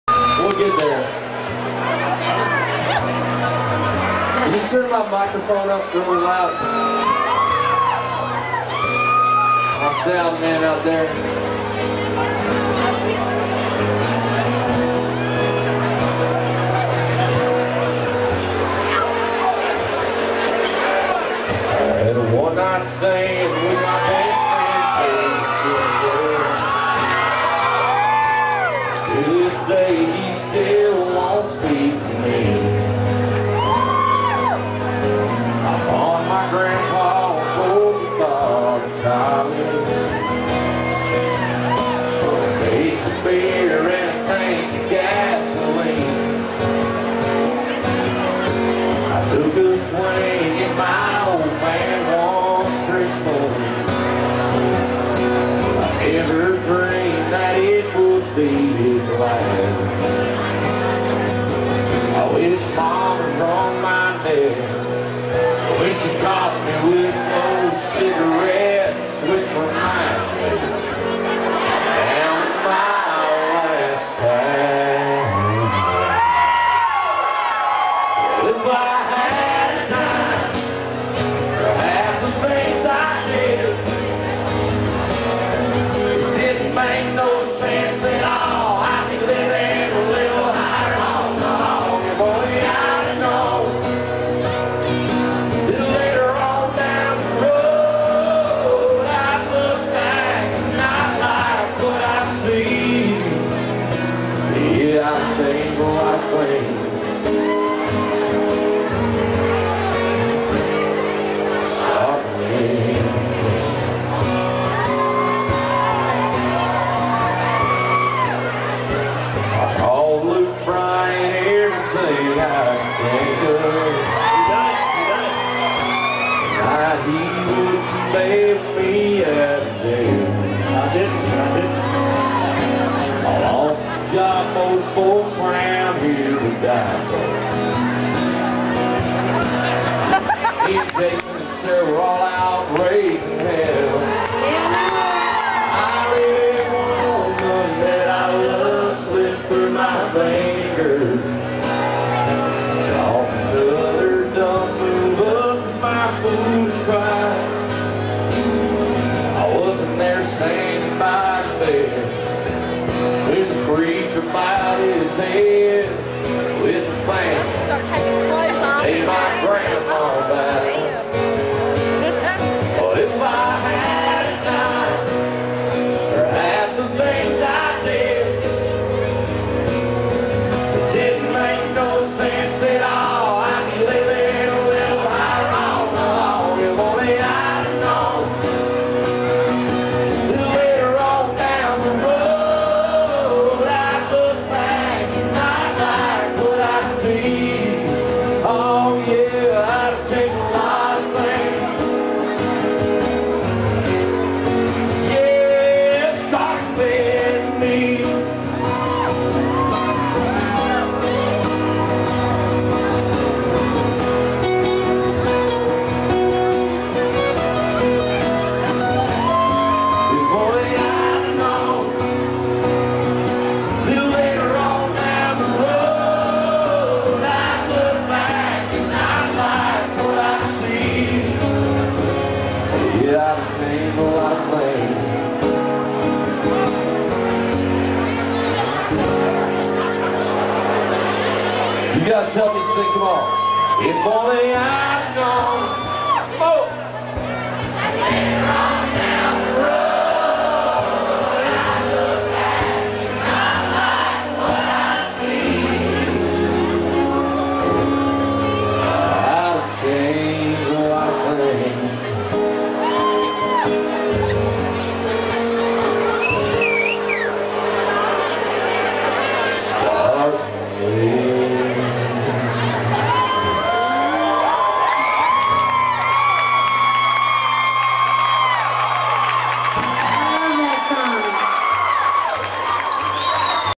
Jake Owen SWM 11-19-2008
Songwriters Showcase Greenville SC Sarah Buxton Jason Michael Carroll Luke Bryan